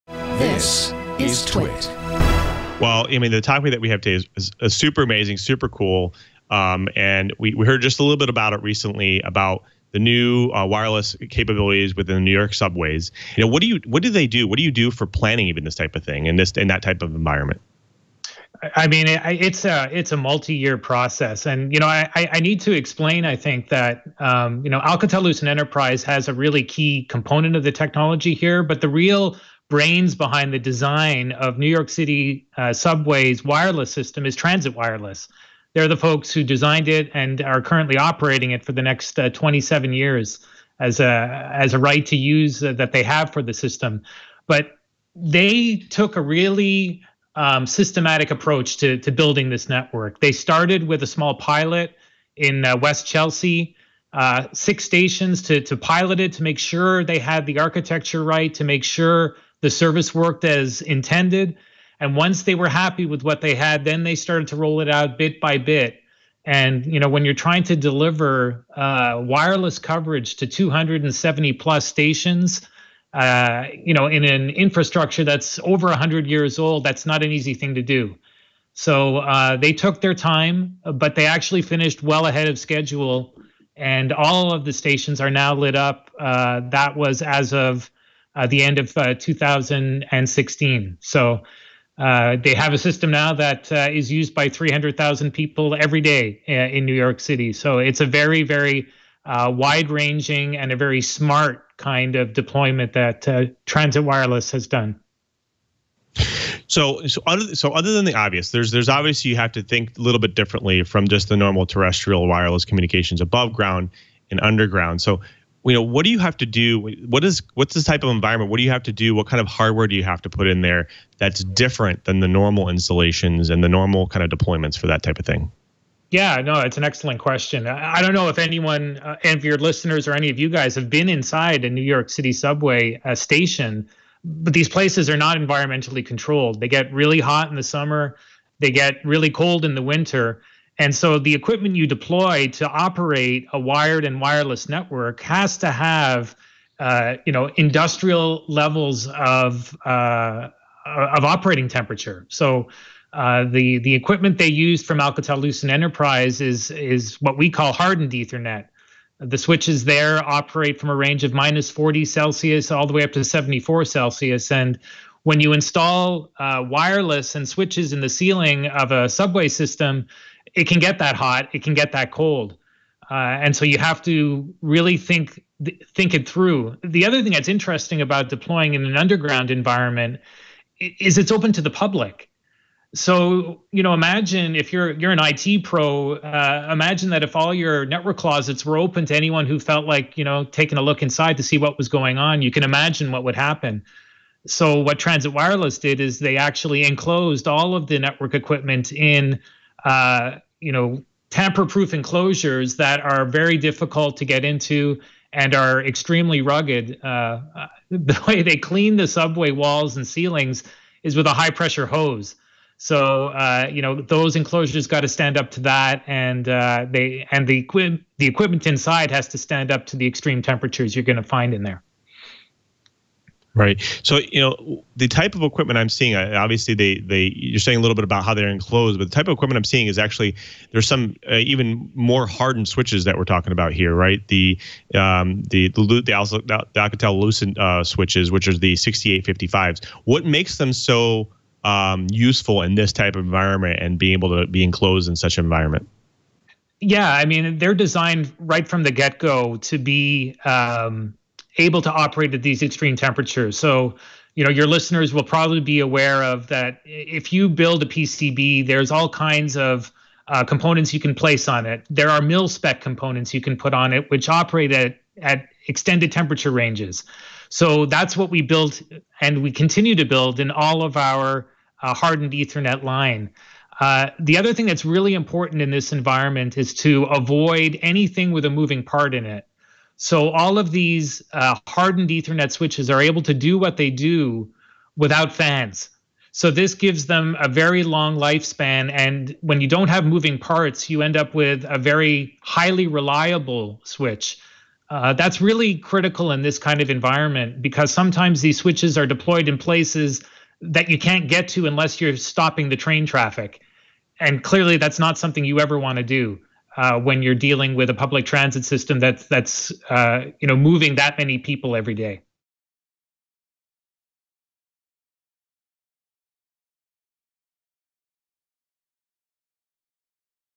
The Hardened Ethernet Switches in NYC's Subway
Lou Maresca interviews Rob Hemmerich, Business Developer at Alcatel-Lucent Enterprise, about the hardened ethernet switches of the New York City subway.